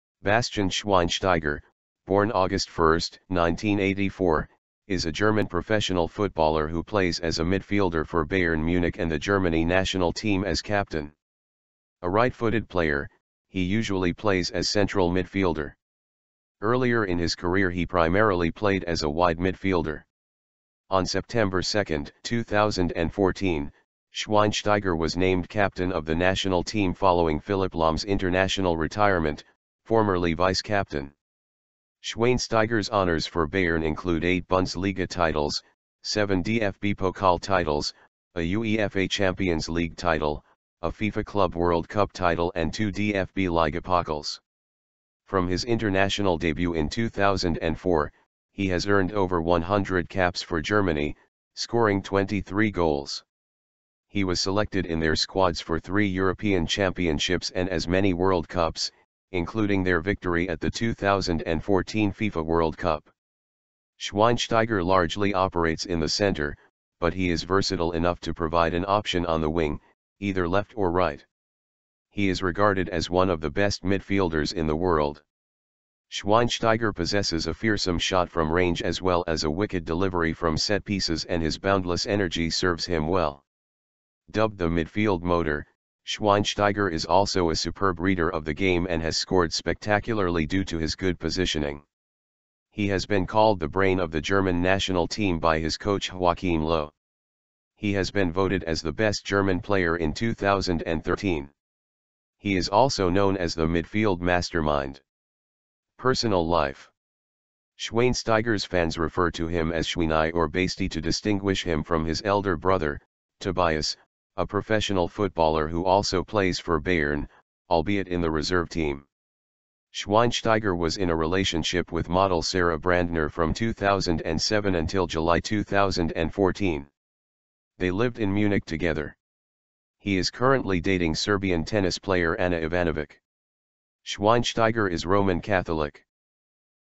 Who Is Bastian Schweinsteiger?
Bastian Schweinsteiger (born 1 August 1984) is a German professional footballer who plays as a midfielder for Bayern Munich and the Germany national team as captain. A right-footed player, he usually plays as central midfielder. Earlier in his career he primarily played as a wide midfielder. On 2 September 2014, Schweinsteiger was named captain of the national team following Philipp Lahm's international retirement (formerly vice-captain).

Schweinsteiger's honours for Bayern include eight Bundesliga titles, seven DFB-Pokal titles, a UEFA Champions League title, a FIFA Club World Cup title and two DFB-Ligapokals. From his international debut in 2004, he has earned over 100 caps for Germany, scoring 23 goals. He was selected in their squads for three European Championships and as many World Cups, including their victory at the 2014 FIFA World Cup.

Schweinsteiger largely operates in the centre, but he is versatile enough to provide an option on the wing, either left or right. He is regarded as one of the best midfielders in the world. Schweinsteiger possesses a fearsome shot from range as well as a wicked delivery from set-pieces and his boundless energy serves him well. Dubbed the "Midfield Motor", Schweinsteiger is also a superb reader of the game and has scored spectacularly due to his good positioning. He has been called "the brain" of the German national team by his coach Joachim Löw. He has been voted as the best German player in 2013. He is also known as the "midfield mastermind".
Personal life

Schweinsteiger's fans refer to him as "Schweini" or "Basti" to distinguish him from his elder brother, Tobias, a professional footballer who also plays for Bayern, albeit in the reserve team. Schweinsteiger was in a relationship with model Sarah Brandner from 2007 until July 2014. They lived in Munich together. He is currently dating Serbian tennis player Ana Ivanovic. Schweinsteiger is Roman Catholic.